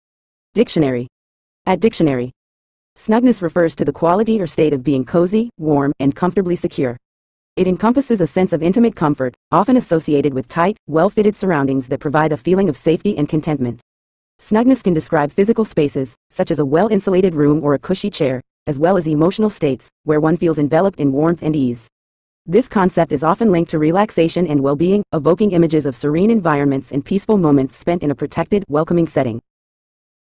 Snugness Meaning In English
Snugness refers to the quality or state of being cozy, warm, and comfortably secure. It encompasses a sense of intimate comfort, often associated with tight, well-fitted surroundings that provide a feeling of safety and contentment. Snugness can describe physical spaces, such as a well-insulated room or a cushy chair, as well as emotional states, where one feels enveloped in warmth and ease. This concept is often linked to relaxation and well-being, evoking images of serene environments and peaceful moments spent in a protected, welcoming setting.